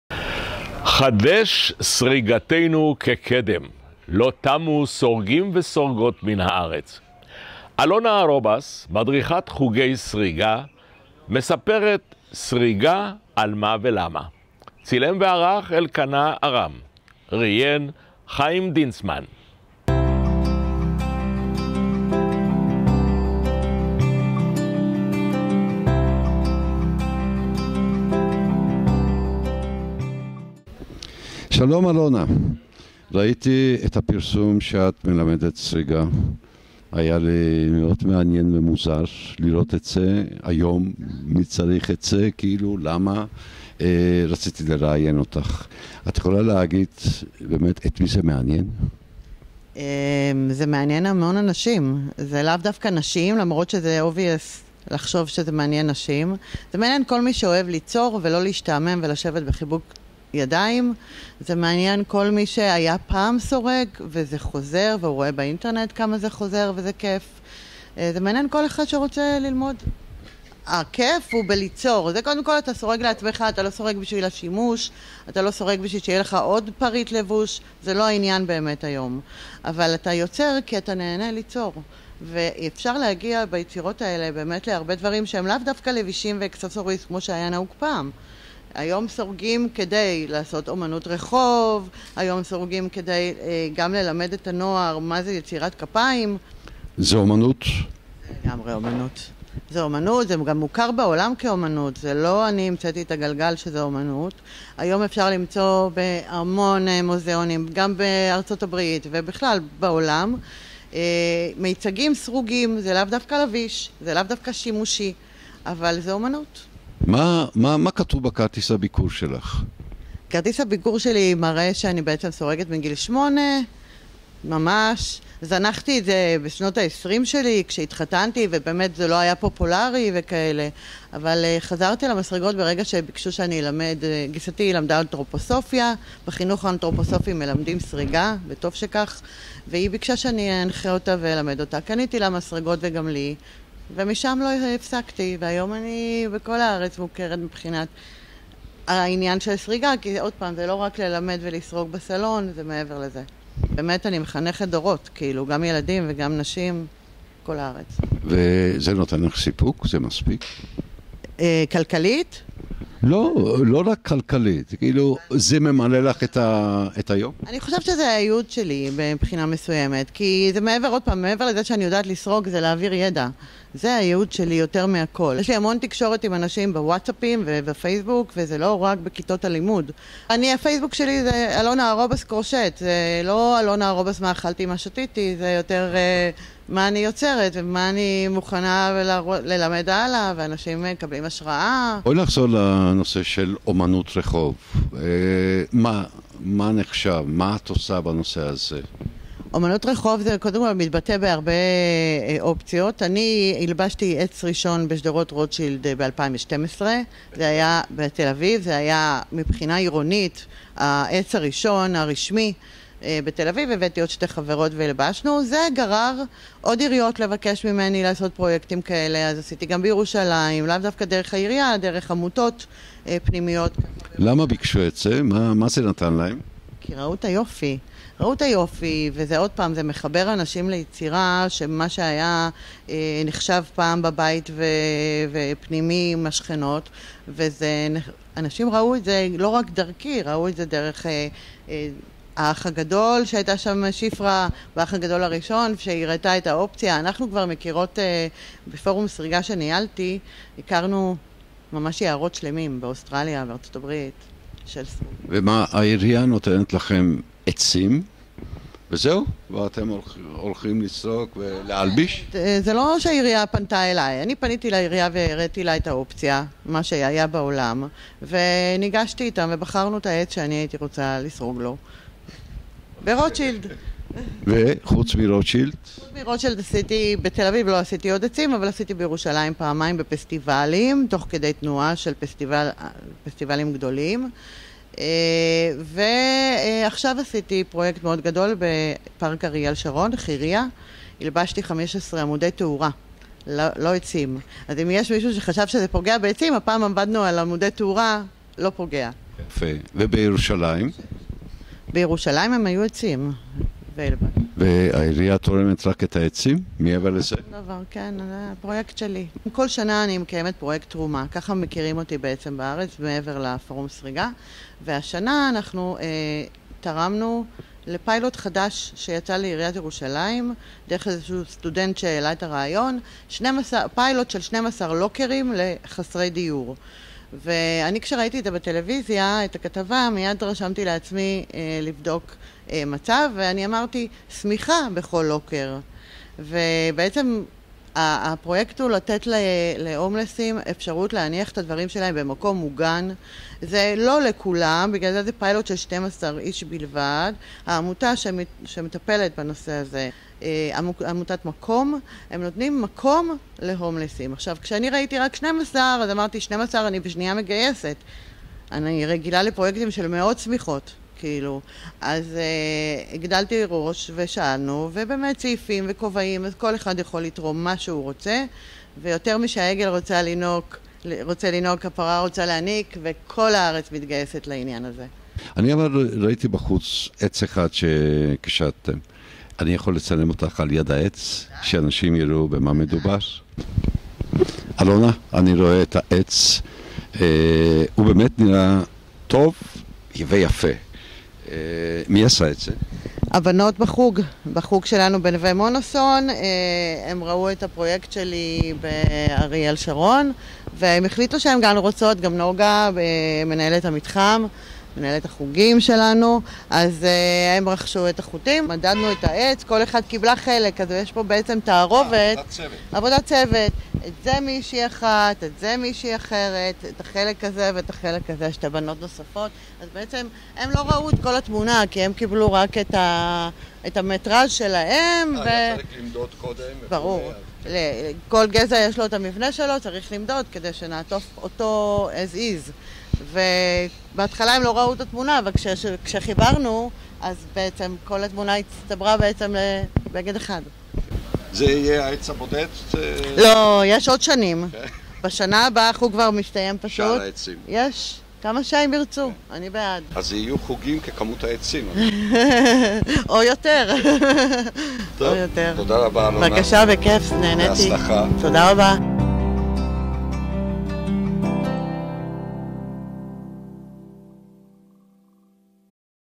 ראיון סריגה 1
חדש סריגתנו כקדם – לא תמו סורגים וסורגות מן הארץ. אלונה ארובס, מדריכת חוגי סריגה, מספרת סריגה על מה ולמה. צילם וערך אלקנה ארם. ראיין חיים דינצמן.